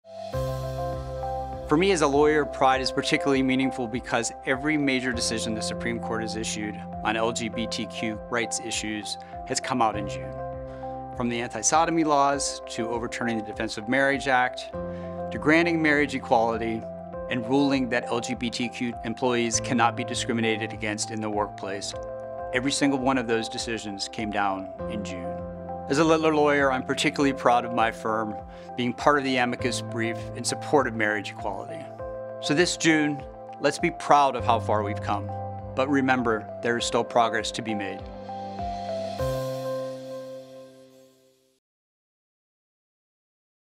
PRIDE 2023: Mark Phillis
In celebration of PRIDEMonth, Littler’s Mark Phillis speaks about the progress in the fight for equal rights for the LGBTQ+ community and the work still to be done.